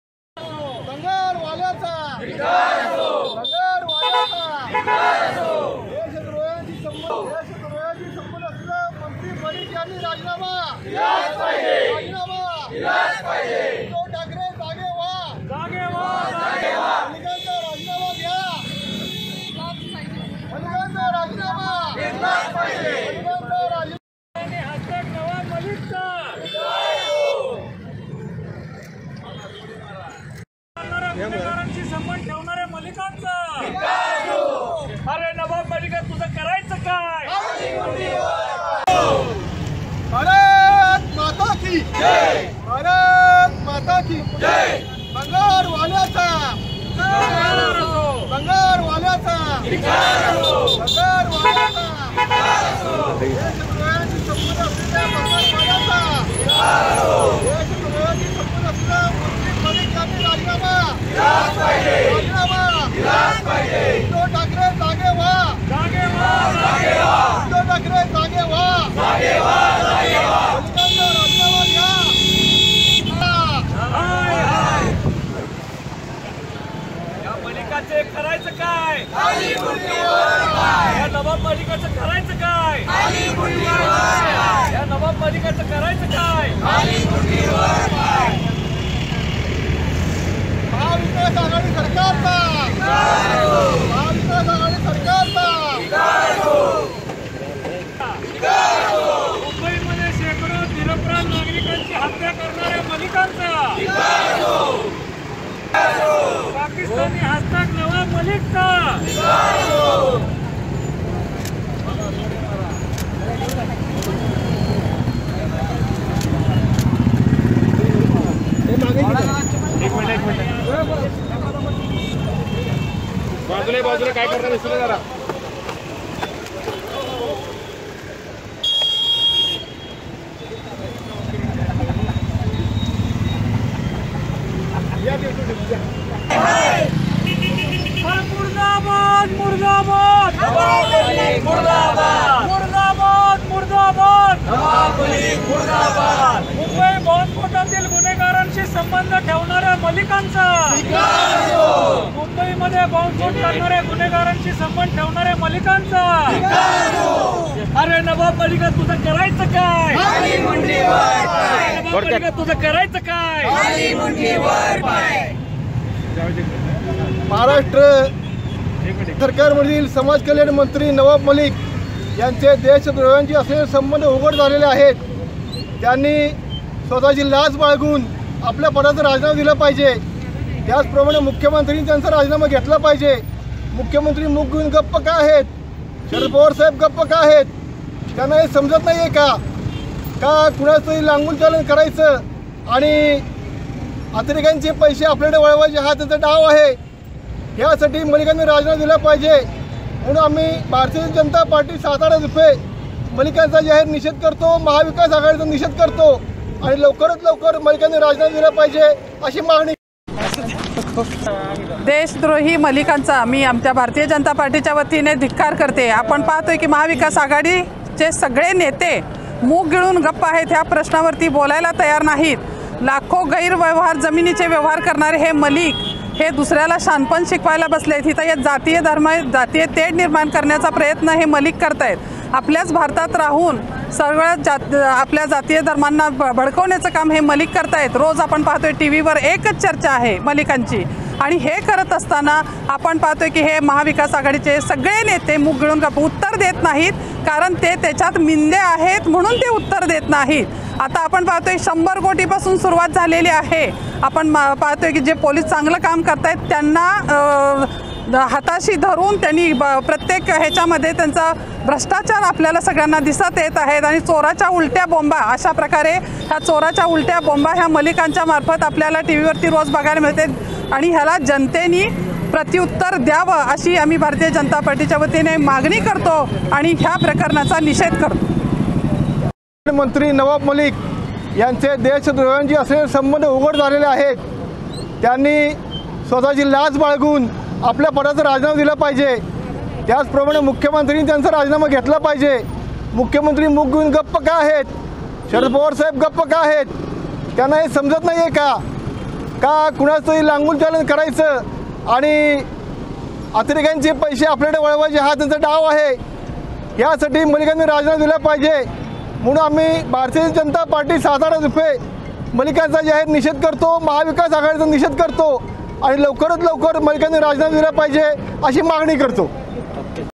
SATARA | भाजपच्या वतीने मंत्री नवाब मलिक यांच्या प्रतिमेला काळे फसत जोडे मारो आंदोलन..
सातारा भाजपच्या वतीने पोवई नाका येथे मंत्री नवाब मलिक यांचा निषेध व्यक्त करत त्यांच्या प्रतिमेला काळे फासून जोडे मारण्यात आले.. यावेळी पोलीस आणि कार्यकर्त्यांच्यात किरकोळ झटपट झाली... नवाब मलिक यांचे देशद्रोह्यांशी संबंध उघड झाले असून त्यांचा मुख्यमंत्री उद्धव ठाकरे यांनी राजीनामा घेतला पाहिजे.. मुख्यमंत्री आणि शरद पवार यावर गप्प का आहेत? असा सवाल भाजपने उपस्थित करत नवाब मलिक यांच्या राजीनाम्याची मागणी केली आहे..

बाईट - विकास गोसावी,शहराध्यक्ष

बाईट --सुवर्णा पाटील,प्रदेश उपाध्यक्ष